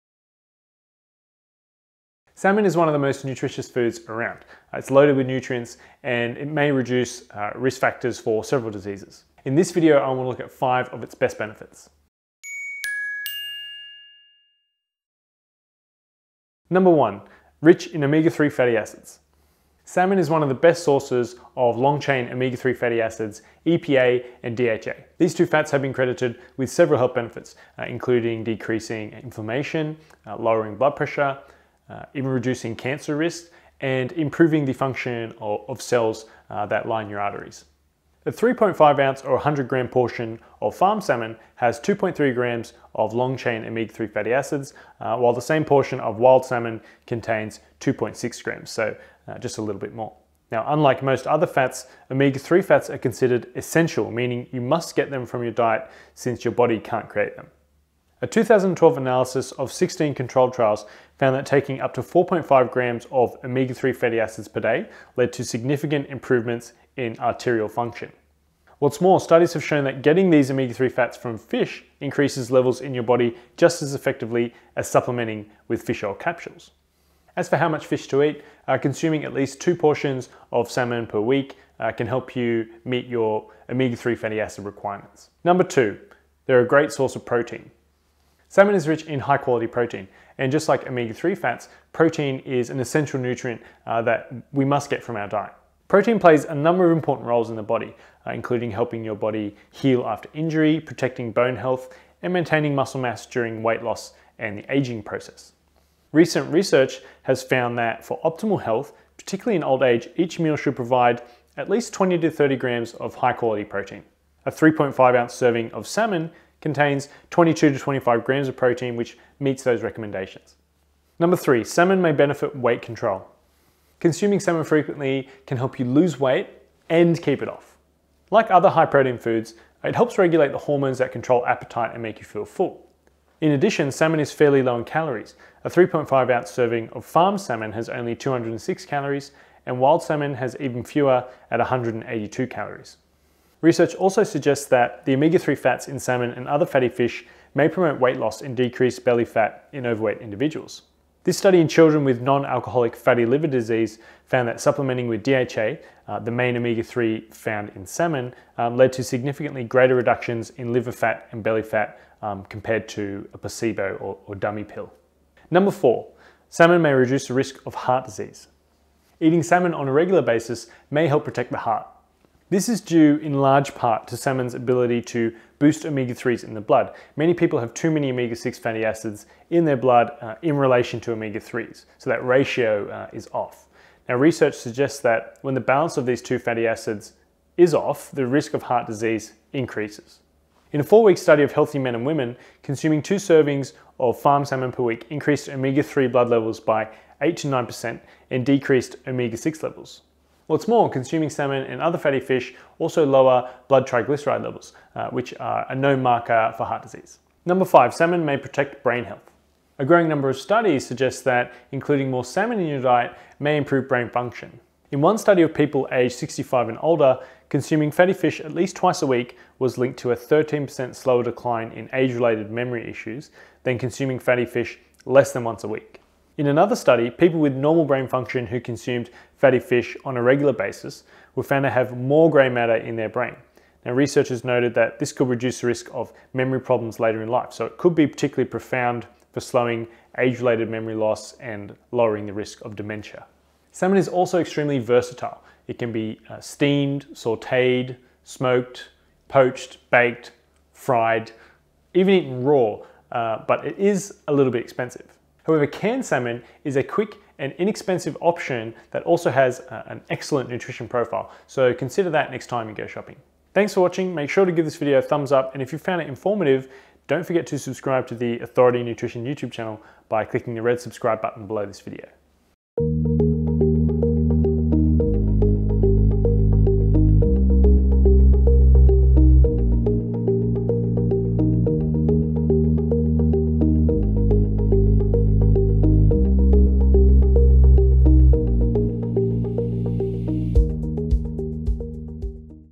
5 Health Benefits of Eating Salmon
Salmon is one of the most nutritious foods around. It's loaded with nutrients and may reduce risk factors for several diseases. In this video I want to look at 5 of its best benefits.